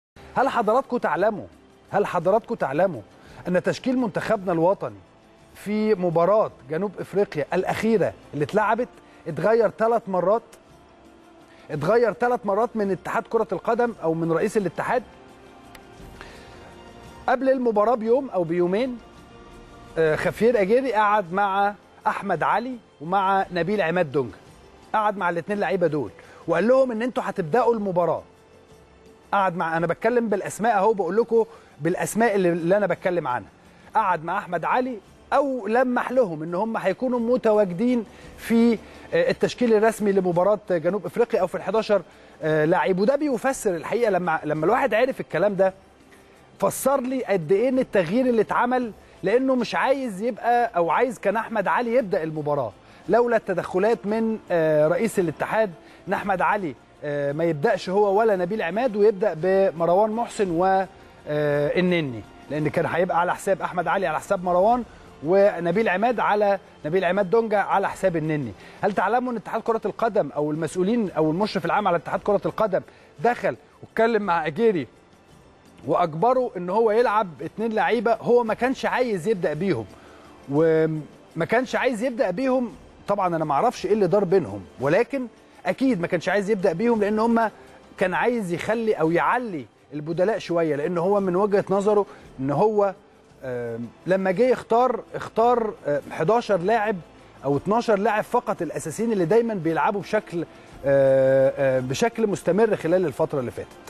اسلام الشاطر يفجر مفاجاة" رئيس الاتحاد تدخل وغير تشكيل المنتخب تلاته مرات فى لقاء جنوب افريقيا "
اسلام الشاطر يفجر مفاجاة على الهواء " رئيس الاتحاد تدخل وغير تشكيل المنتخب تلاته مرات فى لقاء جنوب افريقيا "

التردد: 12341 أفقي.
القمر: نايل سات.
معامل الخطأ: 5/6.
معدل الترميز: 27500